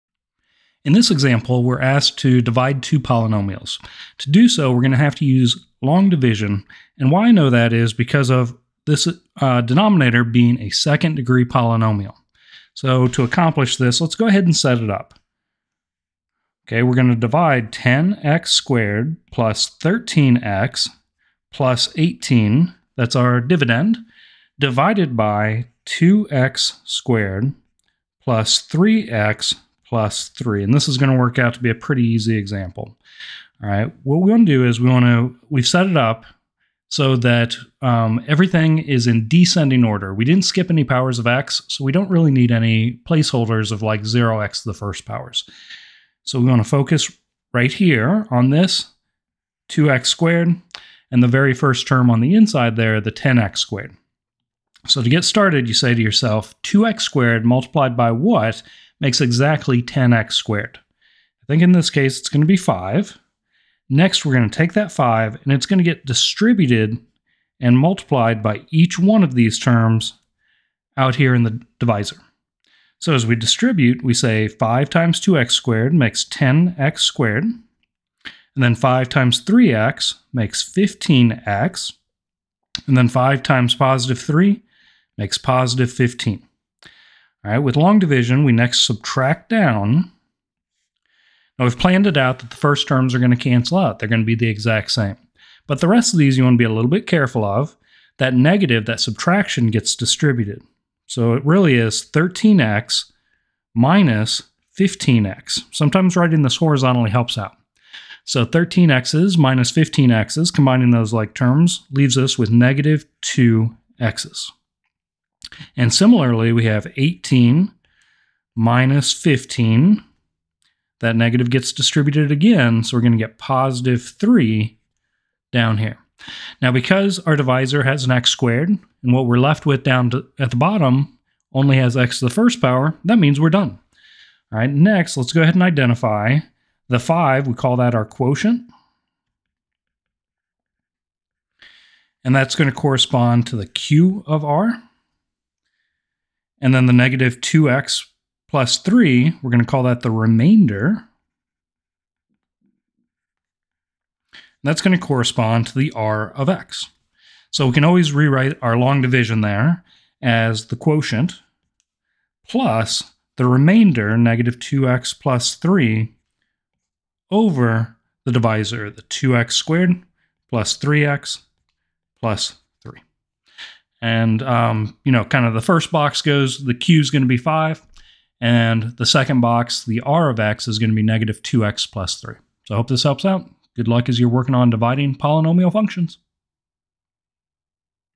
Divide two 2nd degree polynomials | long division
In this math example, we are given two second degree polynomials (quadratic functions) and asked to divide one by the other.  To divide, we walk through the steps to accomplish it using long division of polynomials.  After we complete each, we identify the remainder and quotient and rewrite it in the form q(x)+r(x)/d(x).